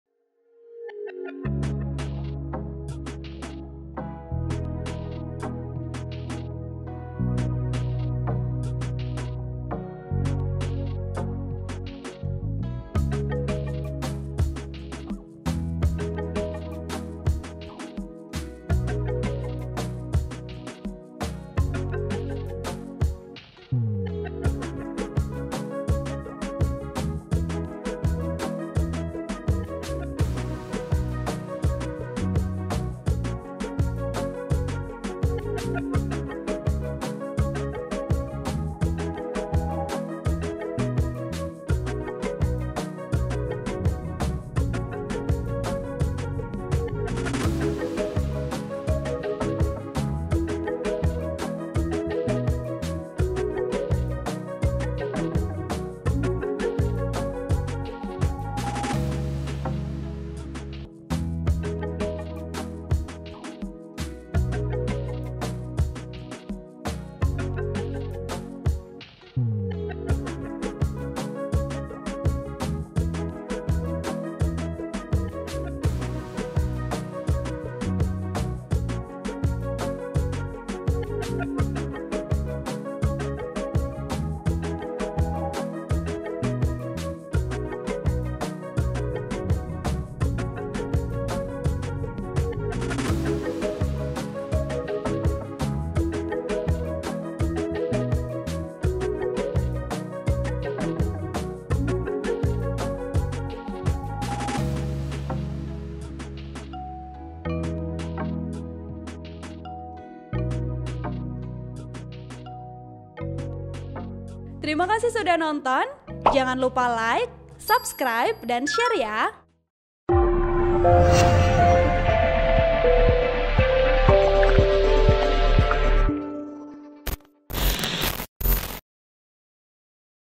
Presiden Jokowi Resmikan Pabrik Pupuk NPK di Aceh Utara
TRIBUN-VIDEO.COM - Presiden Joko Widodo (Jokowi) meresmikan pabrik NPK PT. Pupuk Iskandar Muda (PIM) di Kabupaten Aceh Utara, Jumat (10/2/2023).